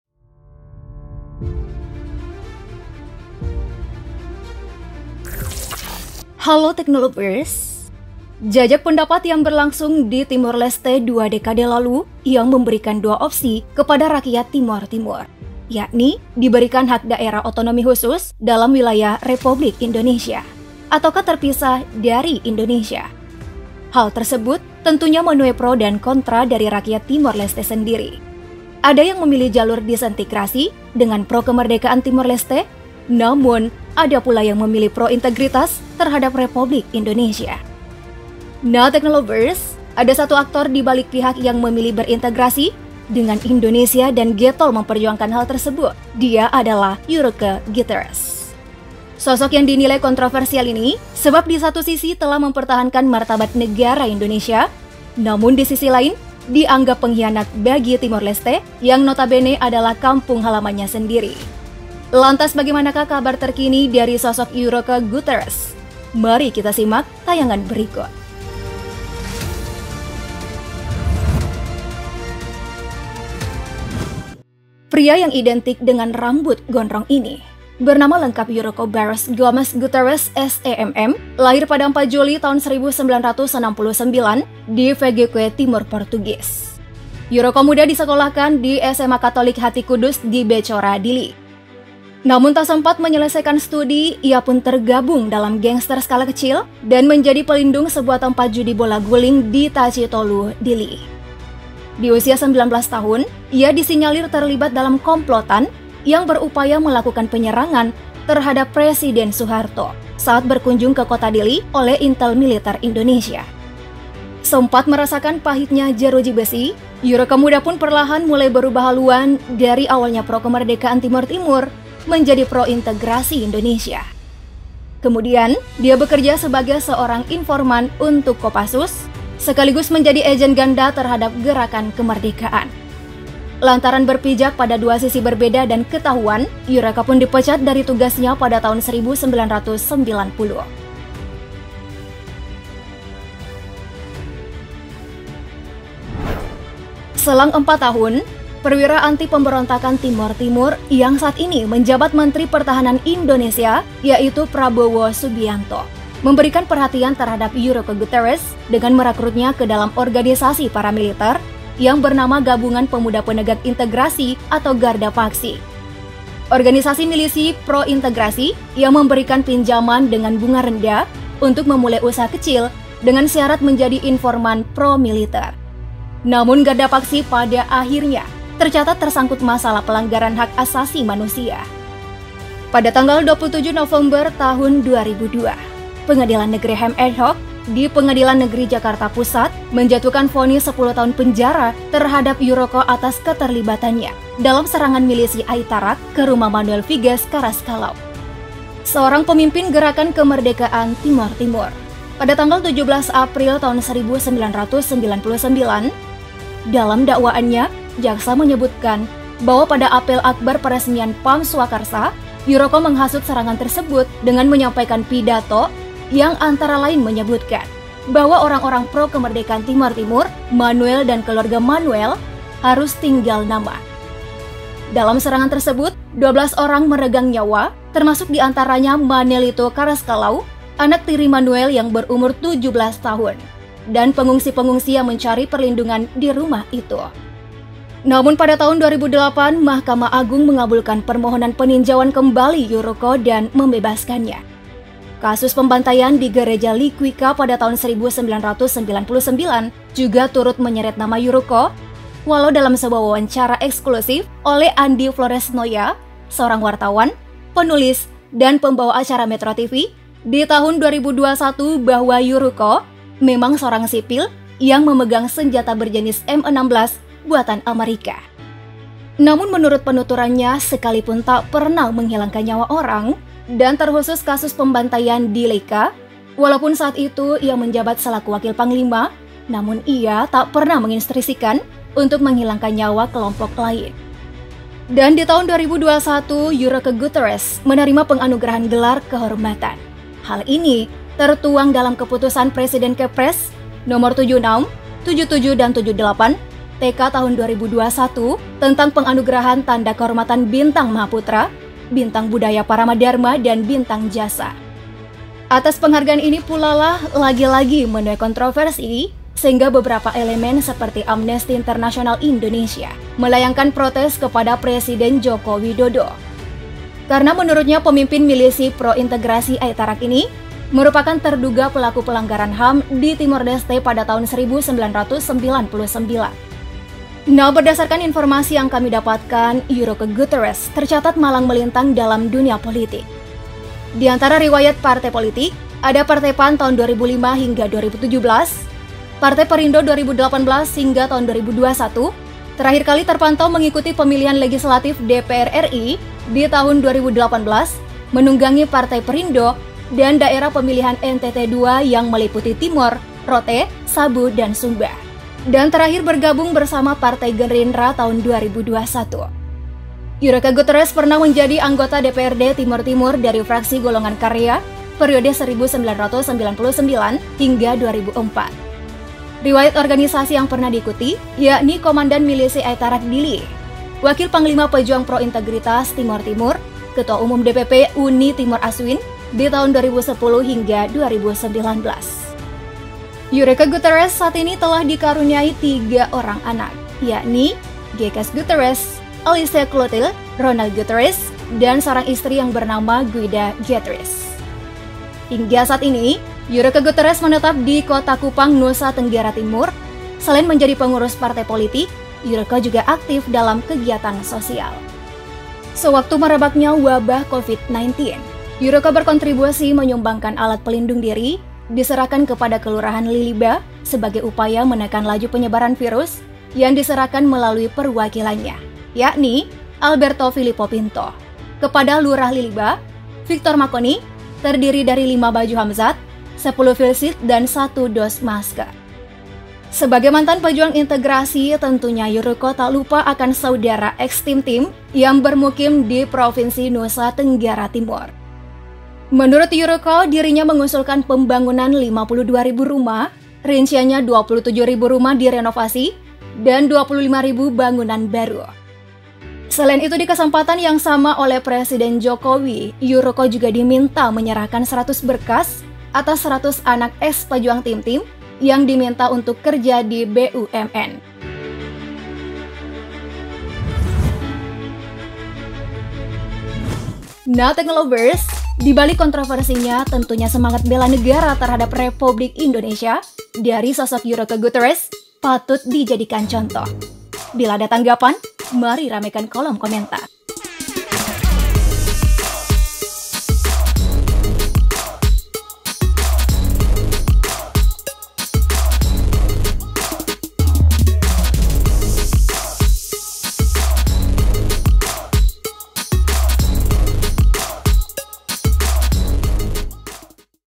MASIH INGAT EURICO GUTERRES YANG MENOLAK INDONESIA MEMERDEKAKAN TIMOR LESTE?Begini Kabarnya Sekarang
HELLO Teknolovers !
Jajak pendapat yang berlangsung di Timor Leste dua dekade lalu yang memberikan dua opsi kepada rakyat Timor Timur yakni diberikan hak Daerah Otonomi Khusus dalam wilayah kesatuan Republik Indonesia ataukah terpisah dari Indonesia. Hal tersebut tentunya menuai pro dan kontra dari rakyat Timor Leste sendiri. Ada yang memilih jalur disentigrasi dengan pro kemerdekaan Timor Leste namun ada pula yang memilih pro integritas terhadap Republik Indonesia. Nah teknolovers ada satu aktor dibalik  pihak yang memilih berintegrasi dengan Indonesia dan getol memperjuangkan hal tersebut, dia adalah Eurico Guterres. Sosok yang dinilai kontroversial ini sebab di satu sisi telah mempertahankan martabat negara Indonesia namun disisi lain dianggap pengkhianat bagi Timor Leste yang notabene adalah kampung halamannya sendiri.  Bagaimanakah kabar terkini dari sosok Eurico Guterres yuk mari kita simak!